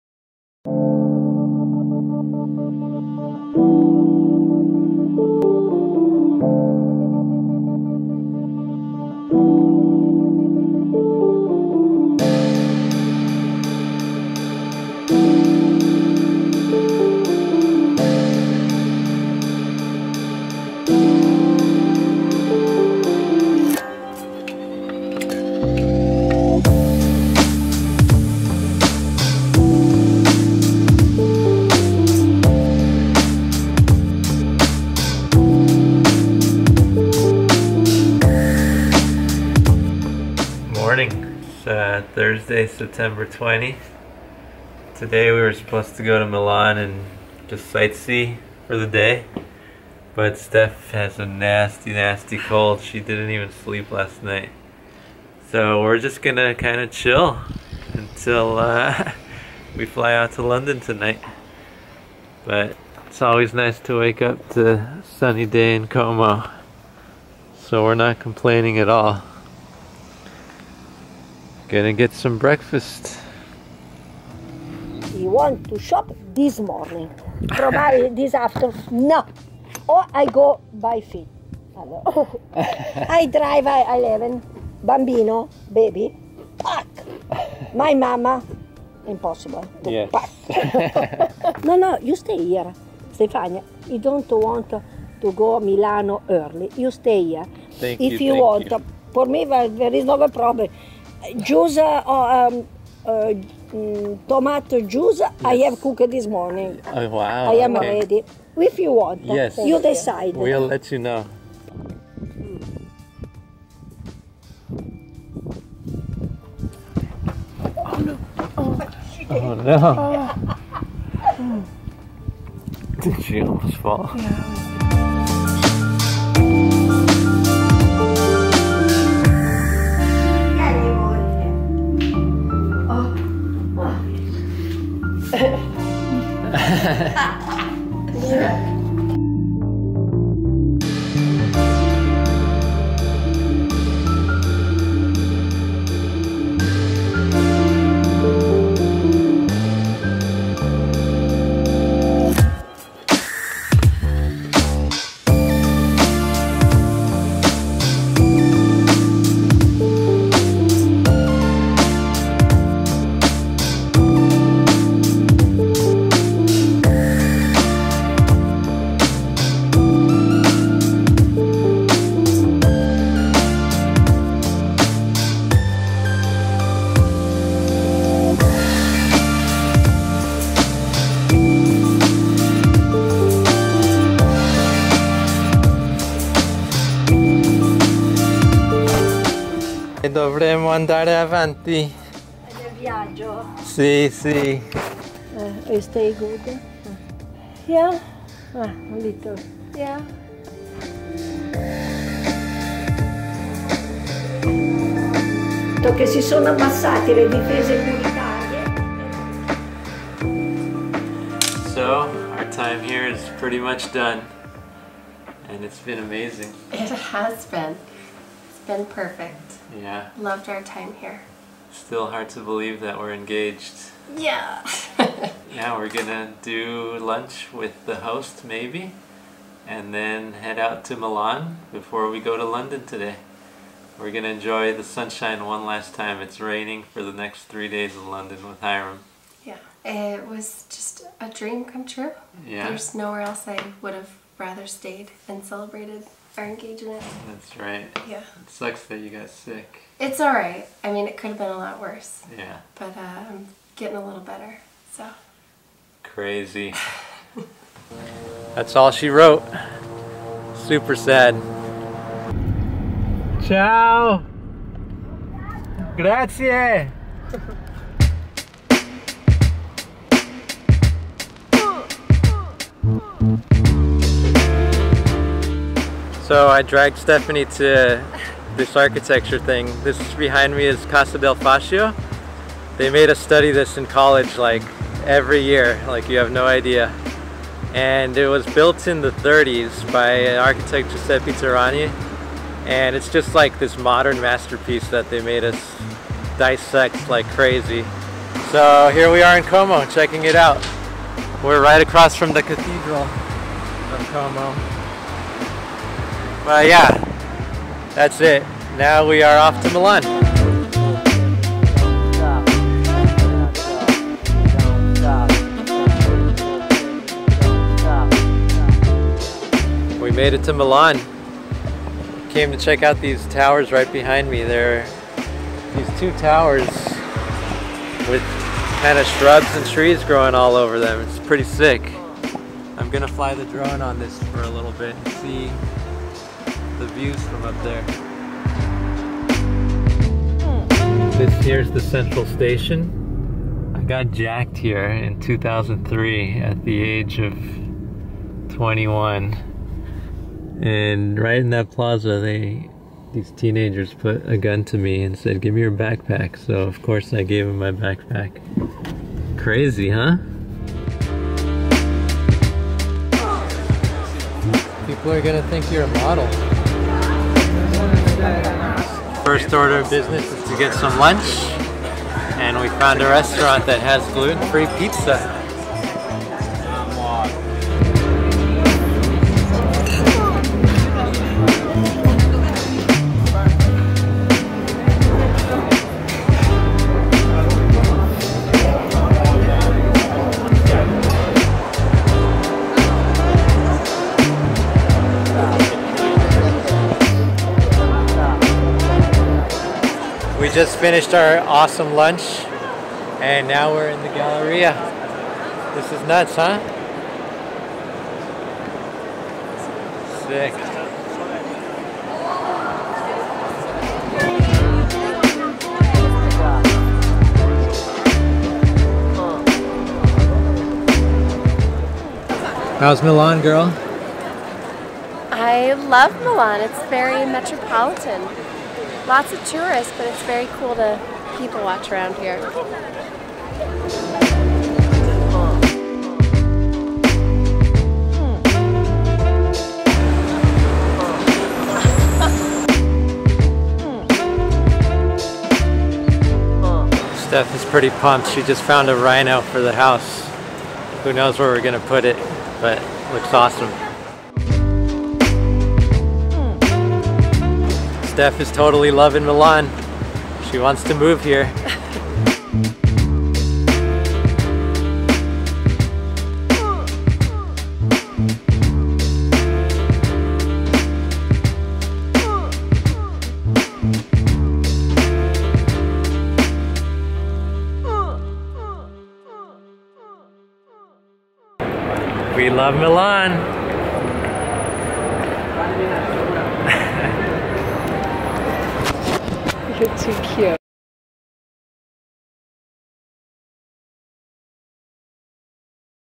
Milan Travel Vlog
We start the day off at our B&B in Cernobbio and make a day trip out of Milan.